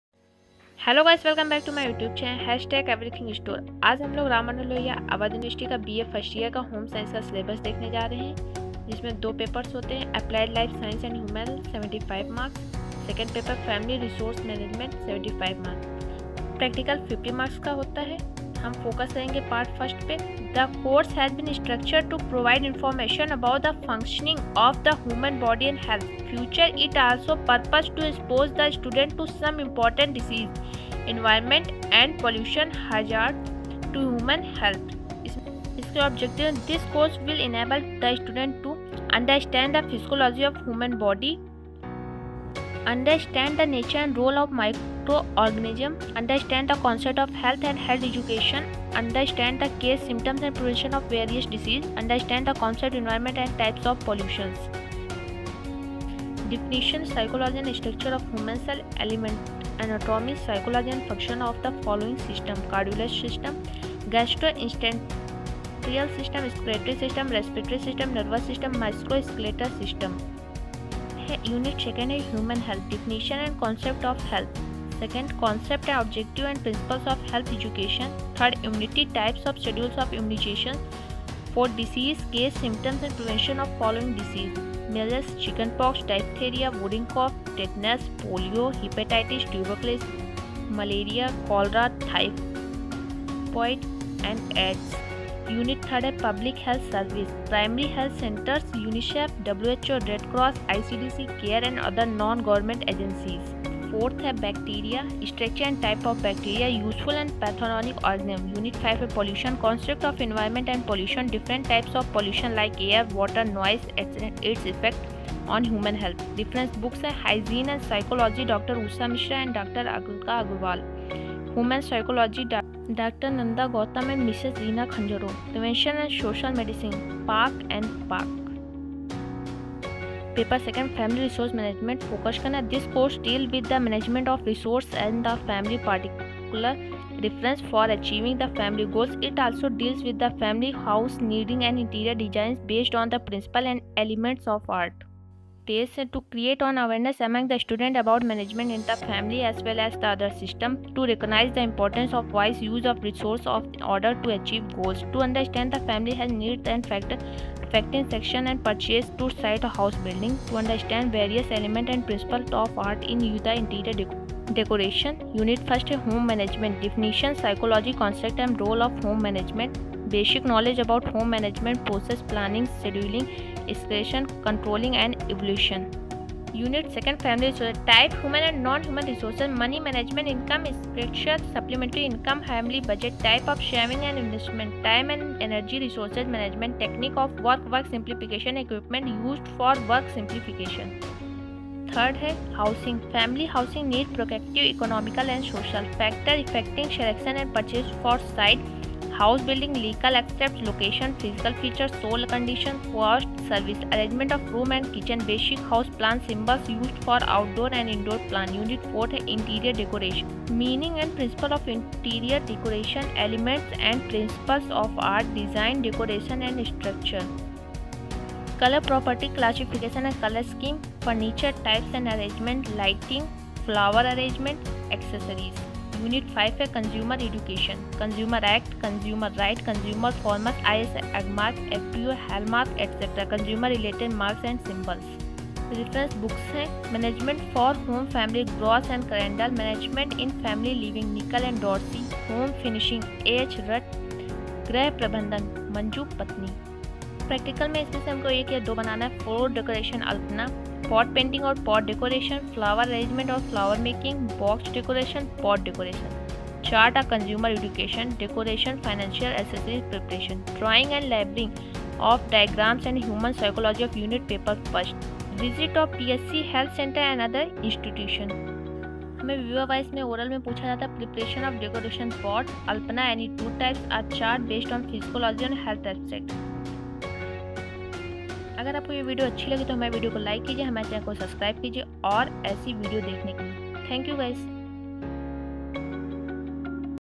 R. m. l. a. u. Home Science B. A. 1st year syllabus //2021-22
Hello friends,

Syllabus of B. A. 1st year Home science ( paper-1 and paper - 2 ) Ram Manohar Lohiya Awadh University.